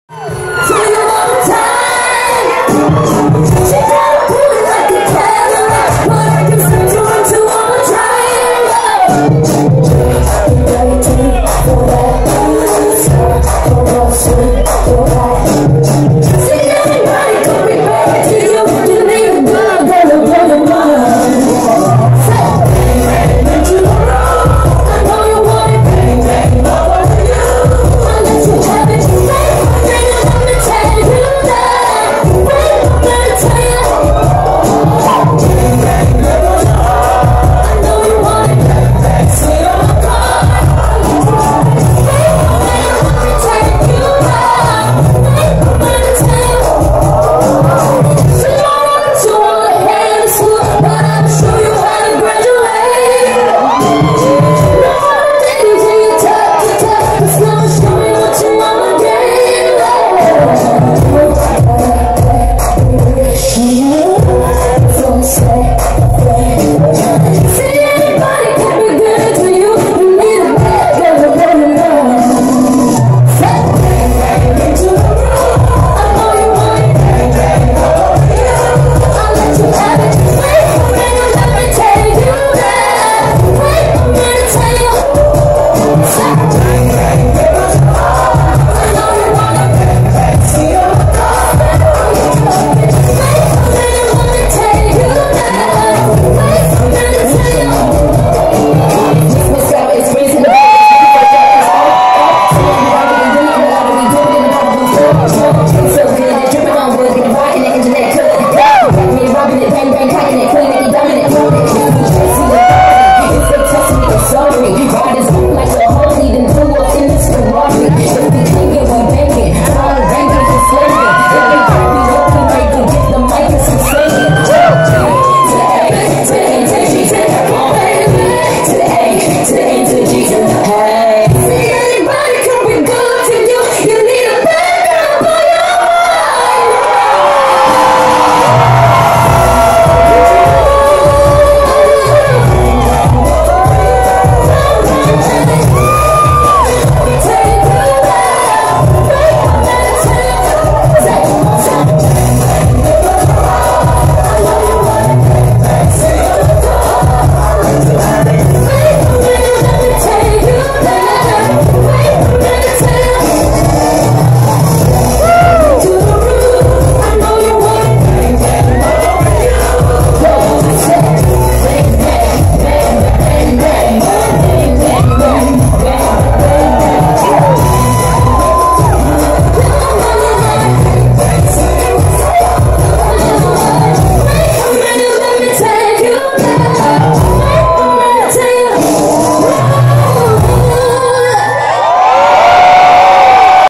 Hyolyn 효린 Concert NYC Final
Hyolyn 효린 U.S solo concert at Flash Factory in NYC